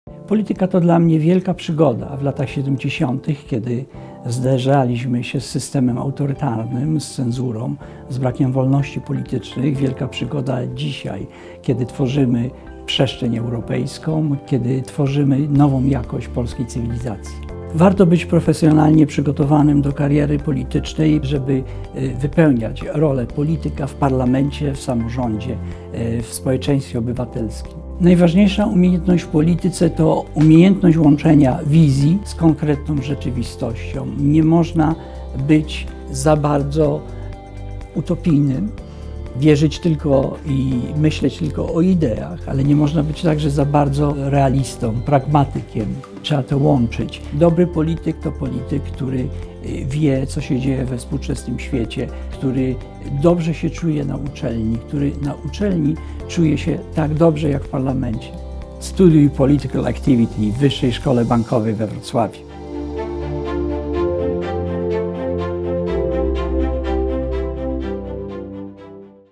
Józef Pinior o polityce i o tym, dlaczego warto być politykiem
Józef Pinior – polski polityk i nauczyciel akademicki, działacz opozycji w PRL, od 2004 do 2009 poseł do Parlamentu Europejskiego, senator VIII kadencji. Dla WSB we Wrocławiu opowiada o polityce i o tym, dlaczego warto zostać politykiem.
Marzysz o karierze polityka? Wybierz Political Activity w Wyższej Szkole Bankowej we Wrocławiu. Kształcimy nowoczesnych polityków!